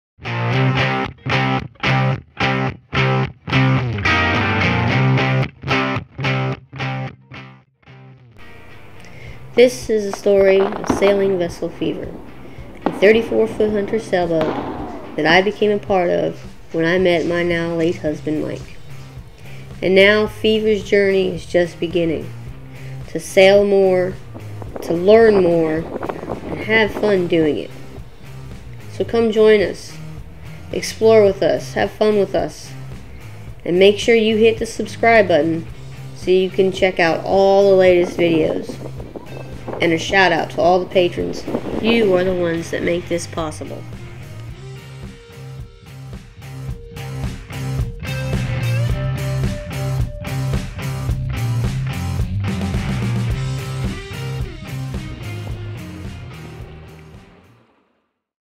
Sailing SV Fevre Intro
Sailing Vessel Fevre, a 34' Hunter Sailboat and her crew. A sailboat that is ready to get back out on the water and so is the crew. Based out of Swansboro North Carolina we are excited to get underway and go sailing for as long as we can.